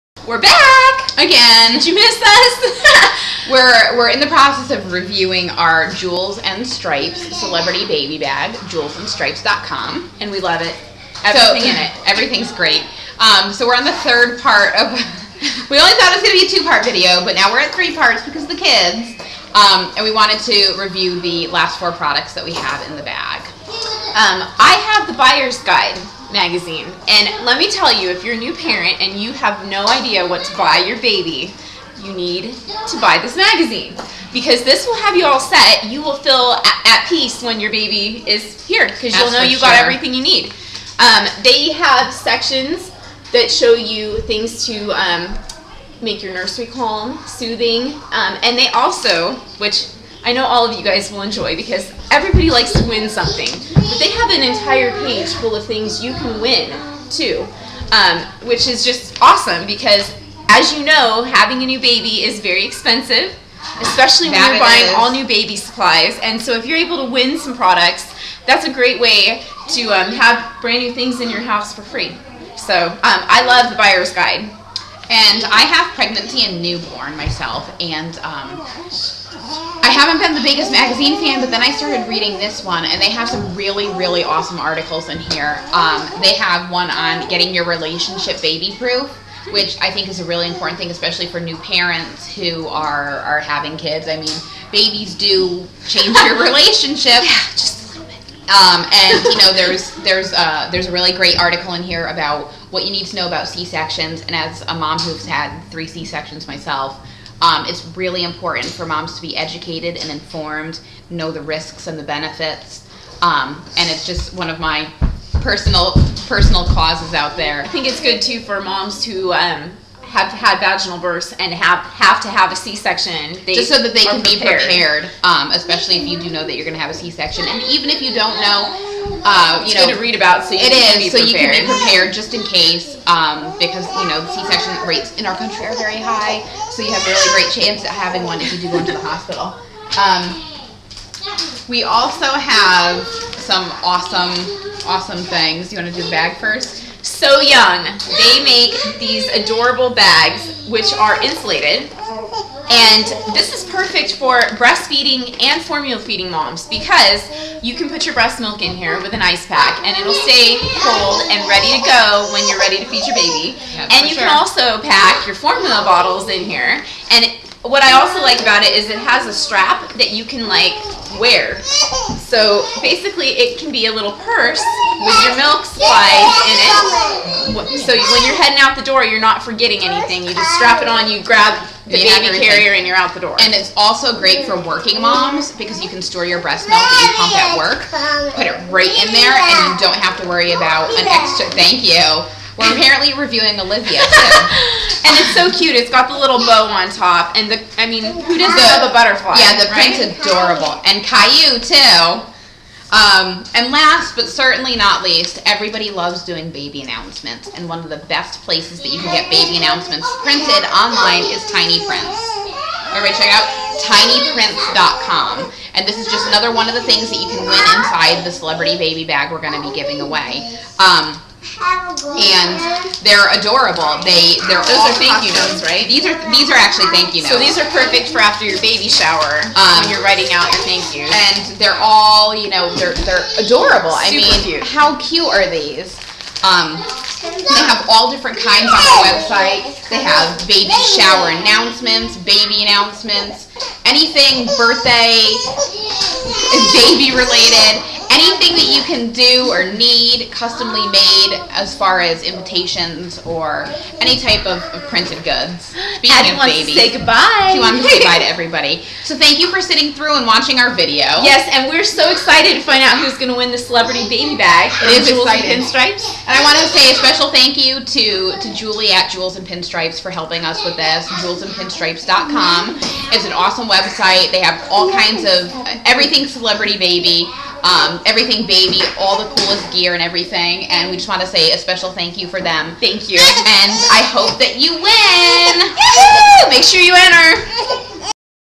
Jewels and Pinstripes Celebrity Baby Gift Bag Review Part 3
Courtney and Danielle from Trendiest Kids on the Blog finish up the Jewels and Pinstripes celebrity baby bag review